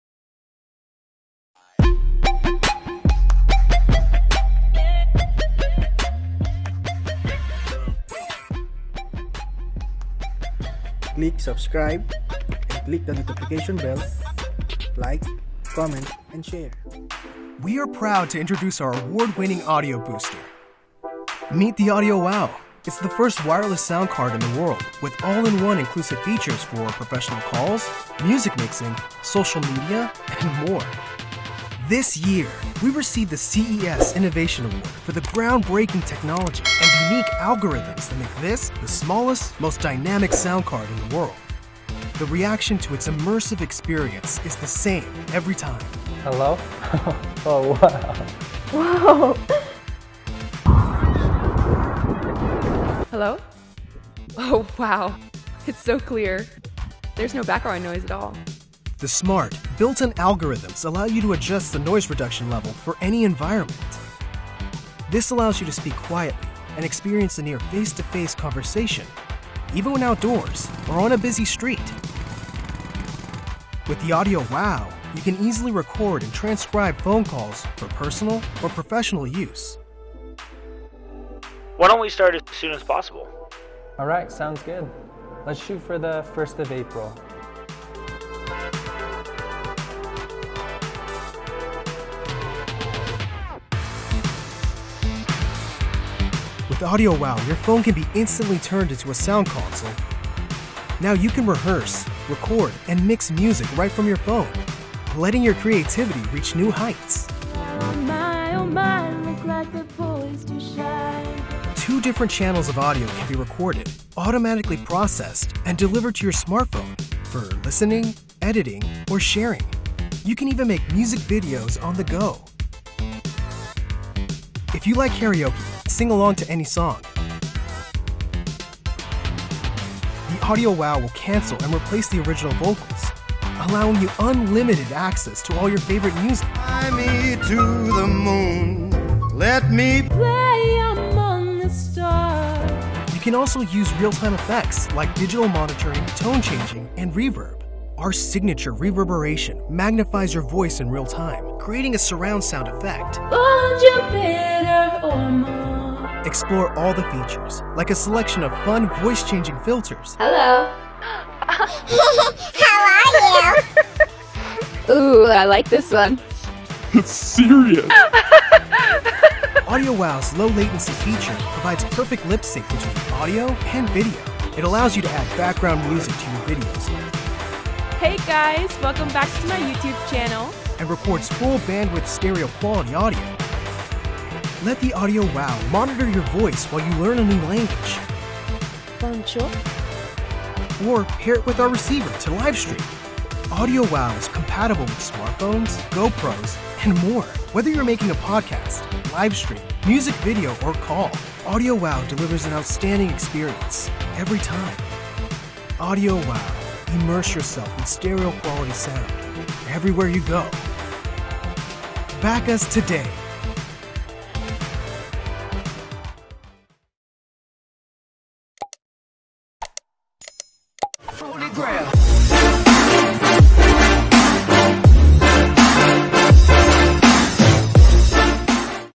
AudioWow: The World’s First Wireless Audio Booster & Mixer
Must-have for content creation, phone calls, karaoke and more. Immerse yourself in clear, studio-quality stereo sound anywhere you go!